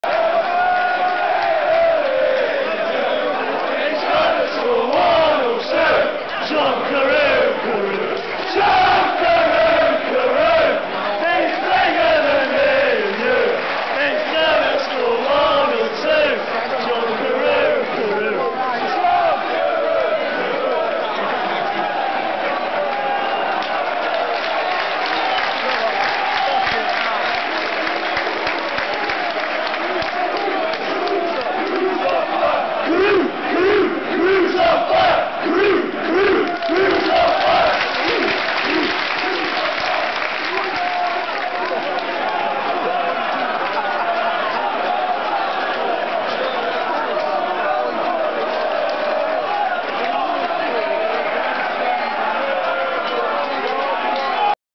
Man United - Villa Carew appreciation! 5/4/09
Villa fans show their appreciation to the Legend that is John Carew!